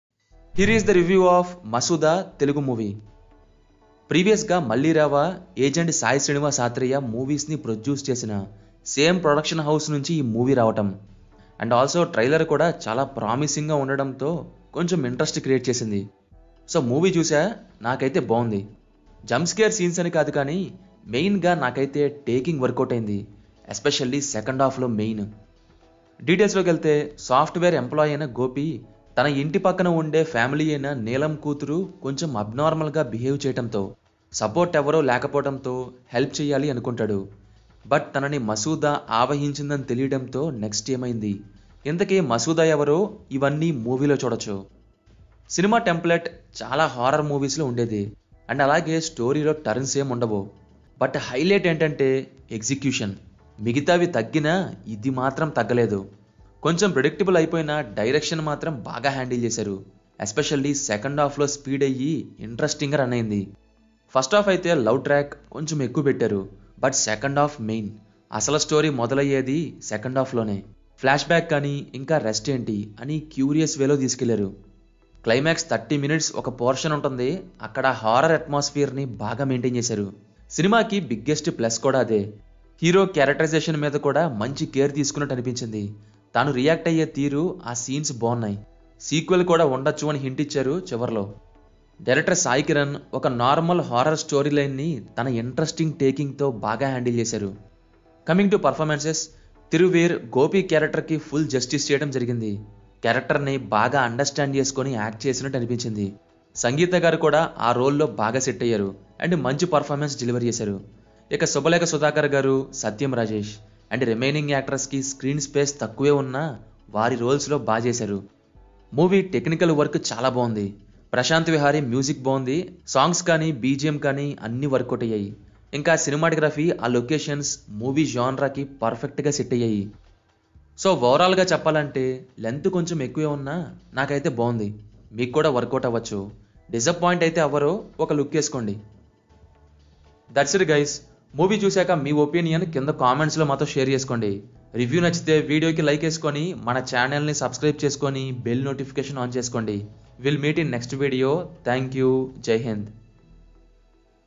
Masooda Movie REVIEW I Sangitha,Thiruveer, SaiKiran | Telugu Movie | Movie Matters
Here is the Review of Masooda telugu Movie starring Sangitha,Thiruveer.

We Movie Matters in this video discussed about the review of Masooda telugu movie

Masooda review,
Masooda  movie review,
Masooda  review,

Movie Matters.